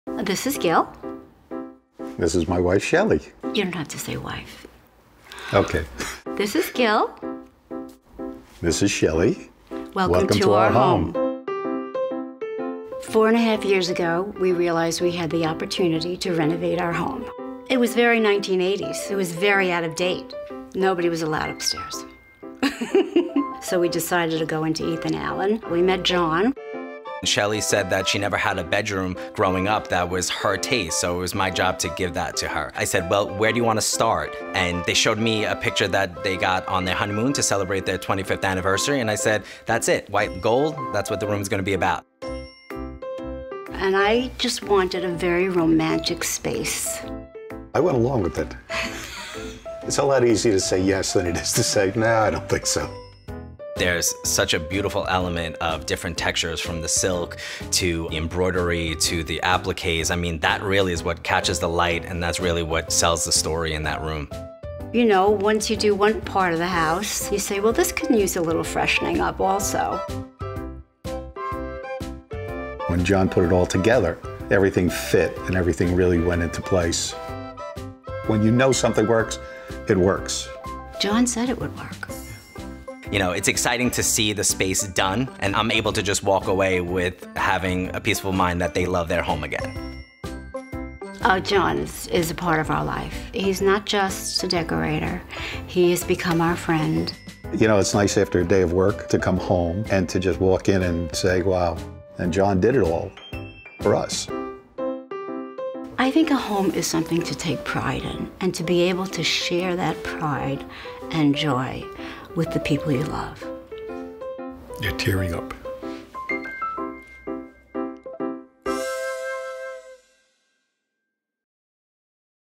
THE START OF A BEAUTIFUL RELATIONSHIP
When John met clients Shelly and Gil, it was love at first design. See how Shelly got the romantic bedroom she'd always dreamed of (Gil likes it, too!).